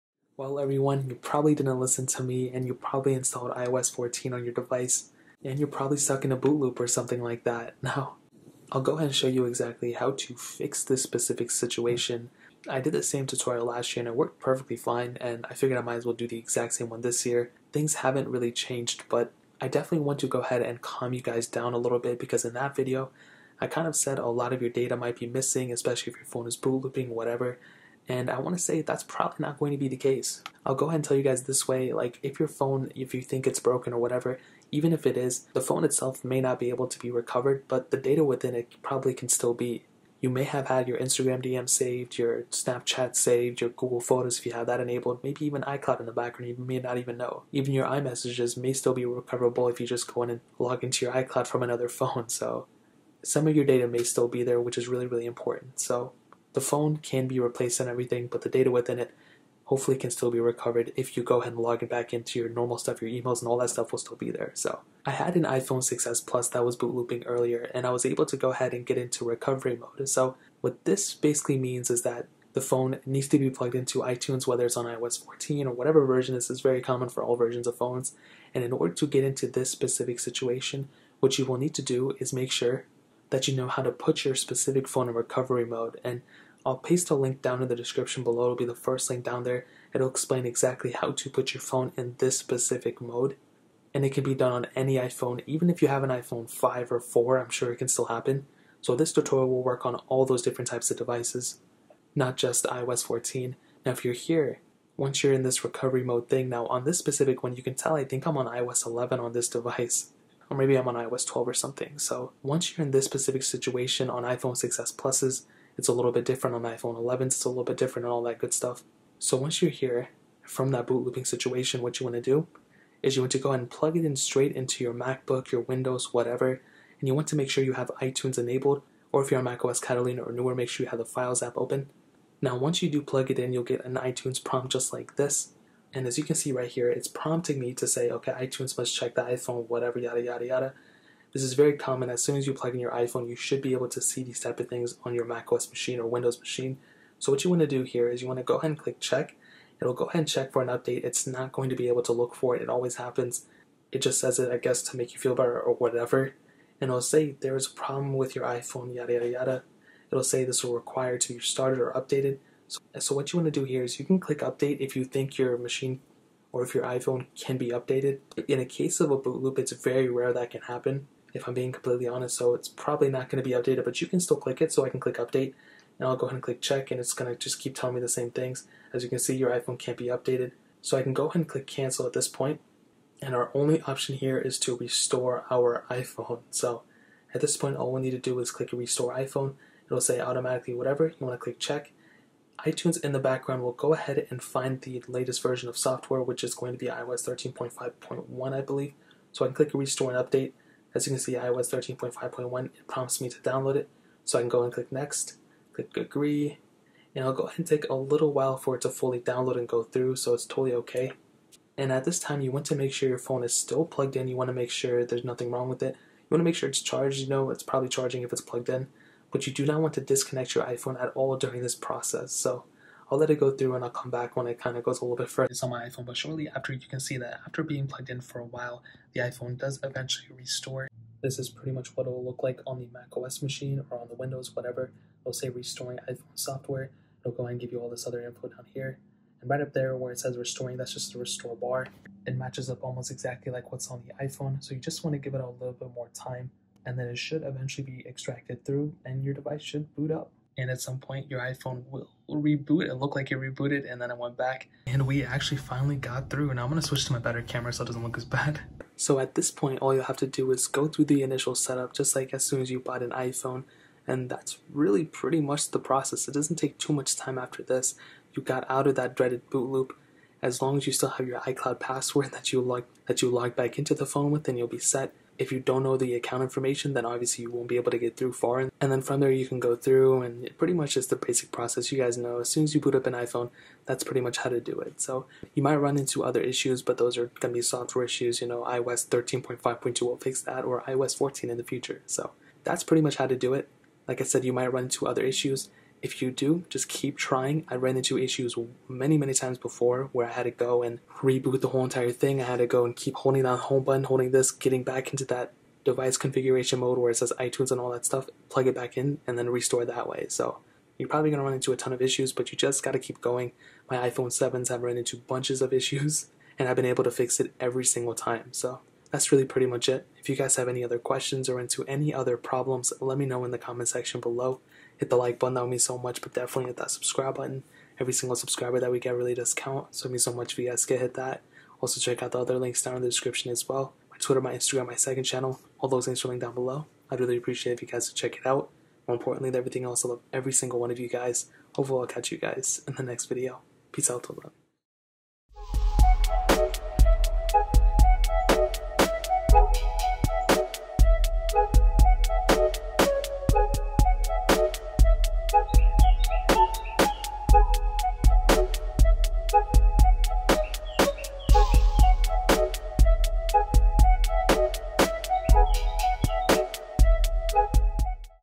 FIX BOOTLOOP On Any iPhone, iPad, iPod (iOS 14, iOS 13, iOS 12)
Written Steps In Case You Don't Want To Hear My Voice

Here is exactly how to fix the infamous Apple logo bootloop that you might be running into when install iOS 14, or any other bootloop issue!